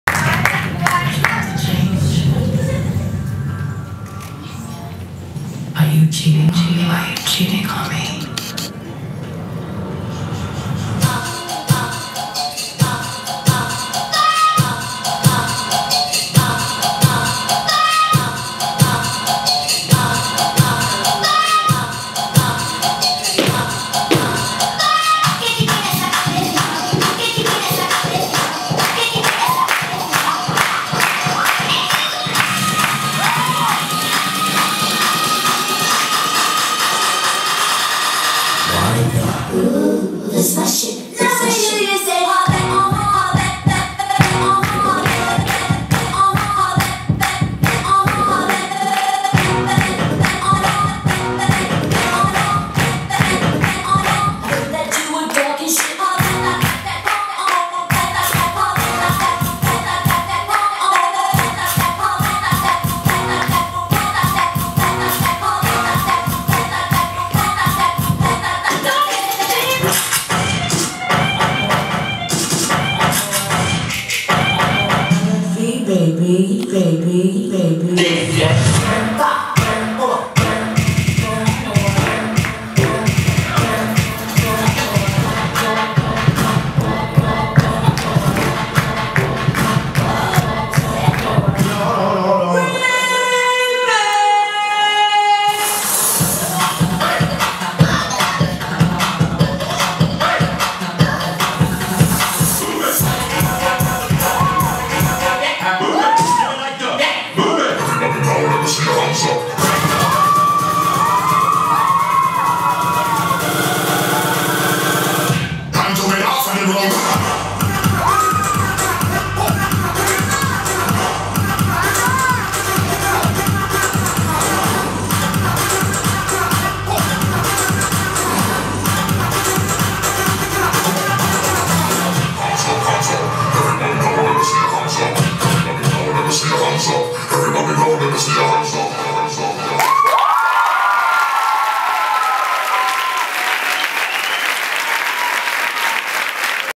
WHY NOT (STREET DANCE) - VI fin de curso SOL DE LUNA
Actuación del grupo de competición WHY NOT (STREET DANCE) de Vanessa Serrano en el sexto fin de curso de la escuela Sol de Luna (24 junio 2017)